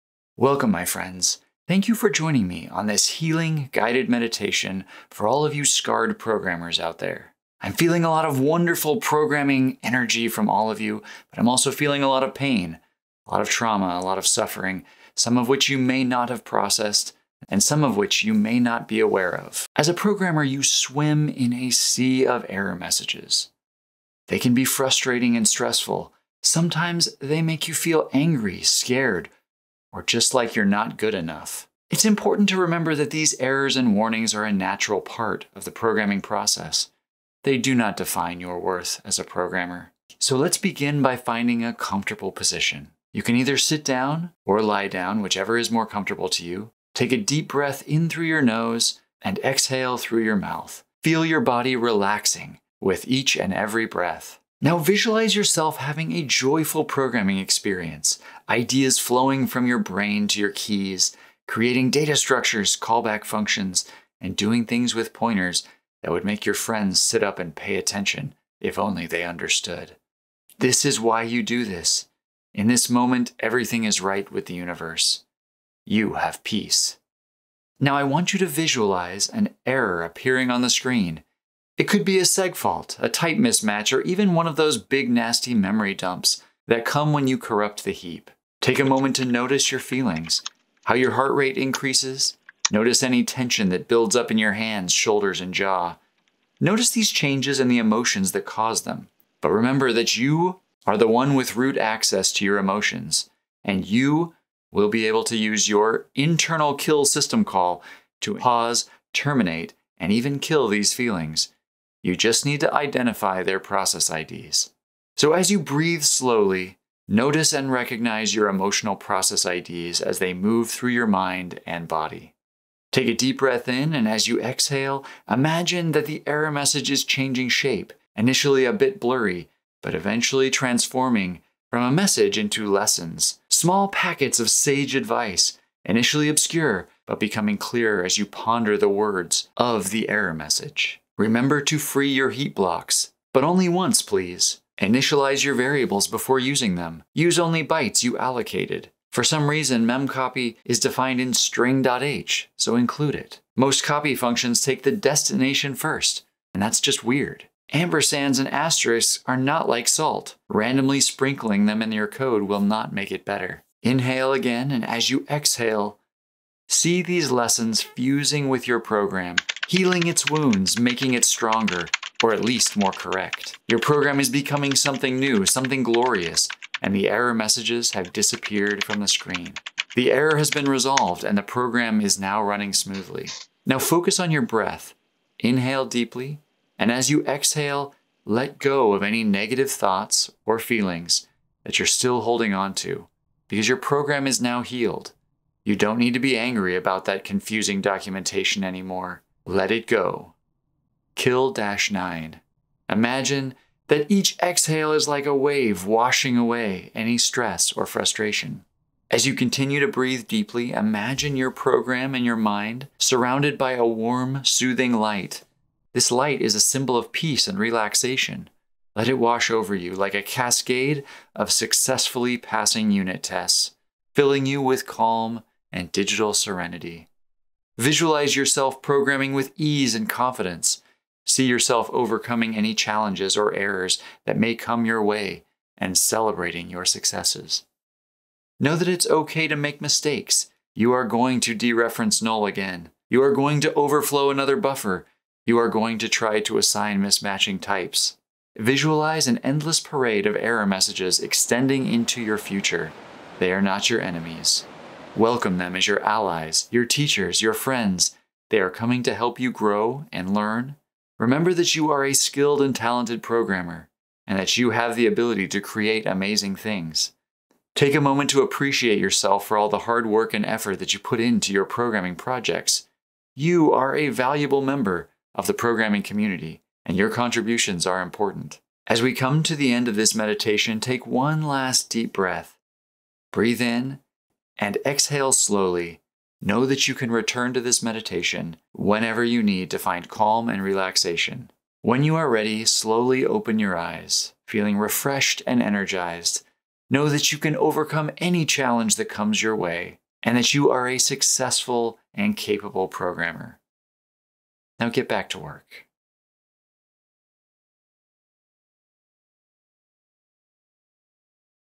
A Guided Meditation for Programmers
Happy April 1st, everyone! I thought we could use a little guided meditation to help you error messages-scarred programmers out there heal a bit.